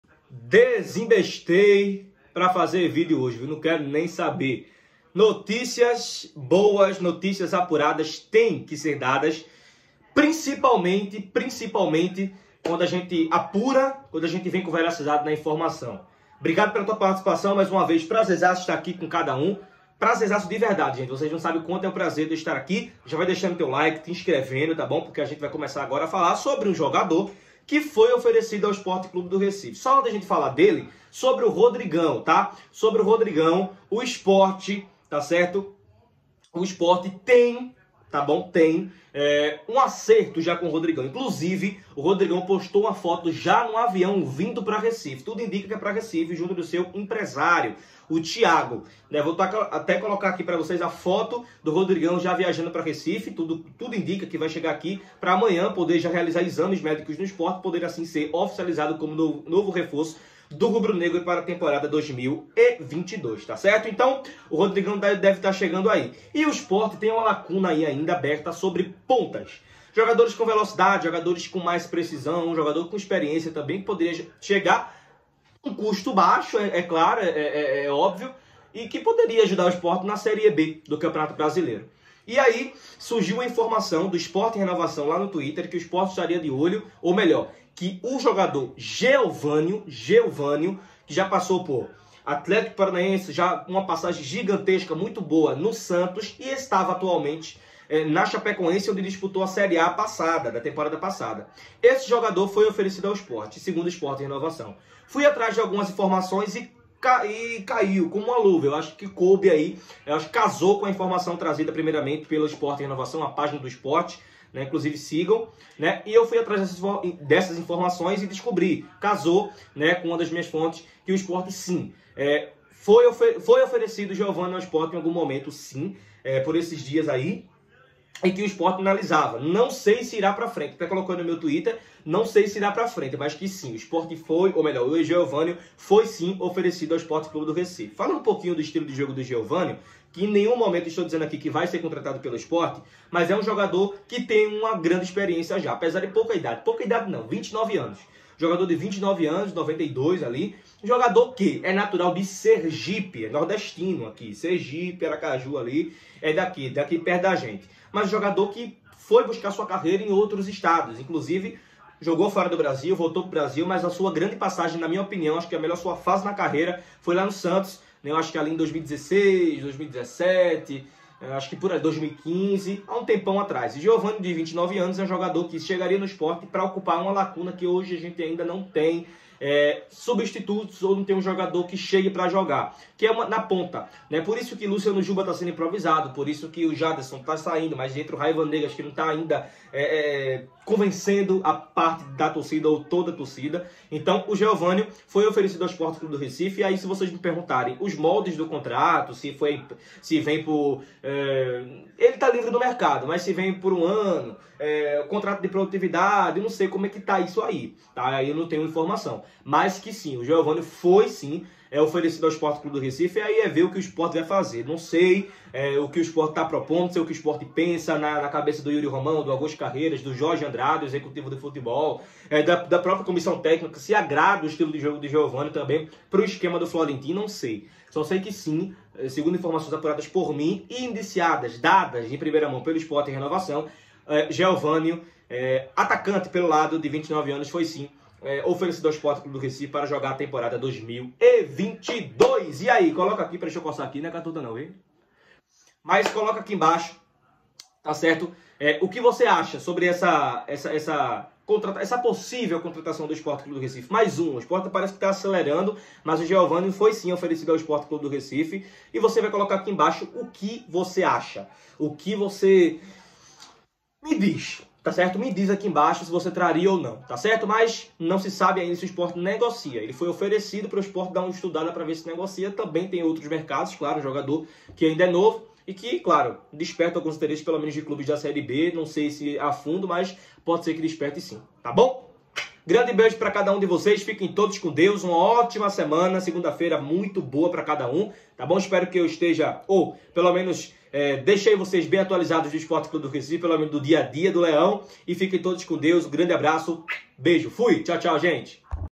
URGENTE🚨: GEUVANIO FOI OFERECIDO AO SPORT - SAIBA TUDO SOBRE O JOGADOR 🦁🚨
Jogador foi oferecido ao Sport 🚨
Saiba tudo sobre o jogador, onde esteve e como está hoje ! Saiba tudooooooooo